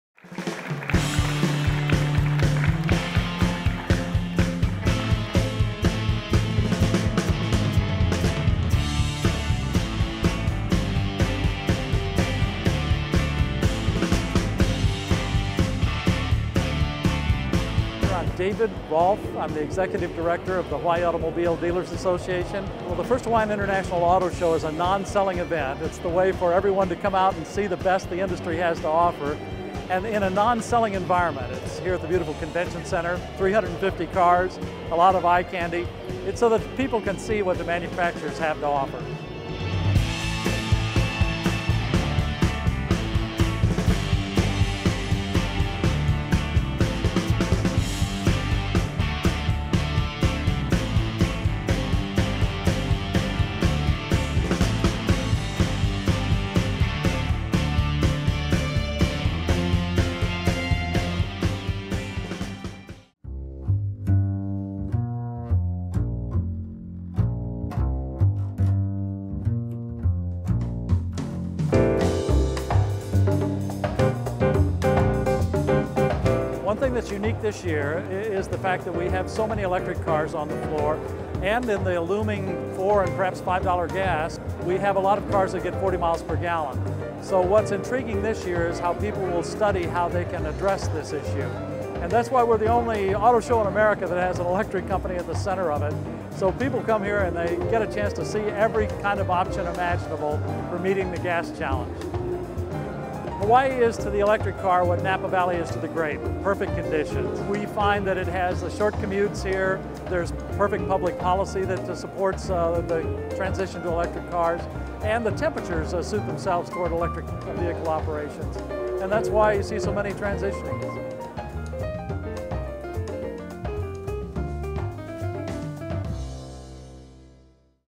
First Hawaiian International Auto Show
:: Designed as a destination to research, compare & try hundreds of new models, the auto show features everything from hybrids and the hottest new coupes & crossovers, to full-size trucks, test drives & appearances...